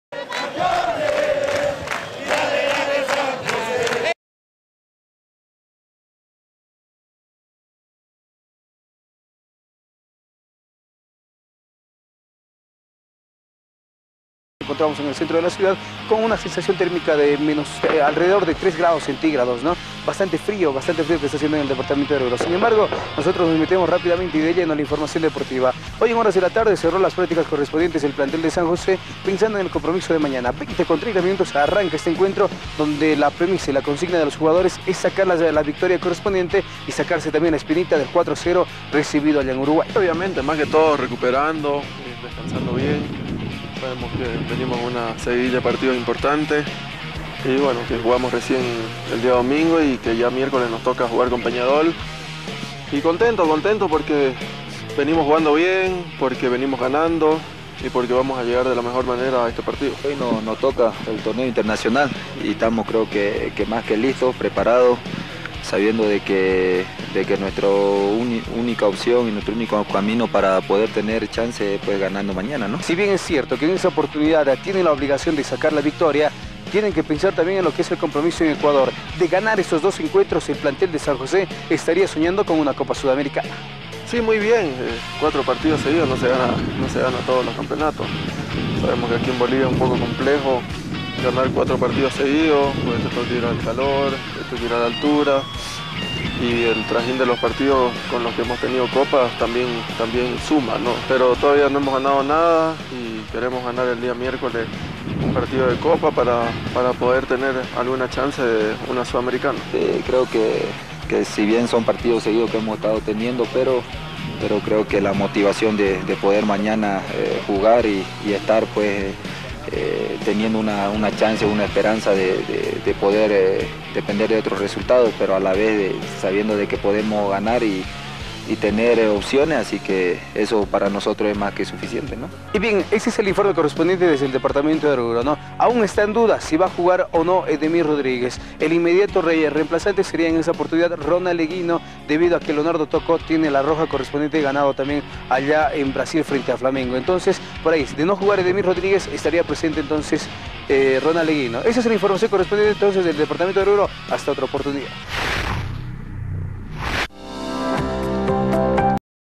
Eguino titular? hablan Lampe y Ovando del partido contra Peñarol
Debido a que Edemir esta con una molestia el titular seria Eguino, ya que Toco fue expulsado y no esta habilitado para este partido, hablan Carlos Lampe y Mario Ovando del partido contra Peñarol!!!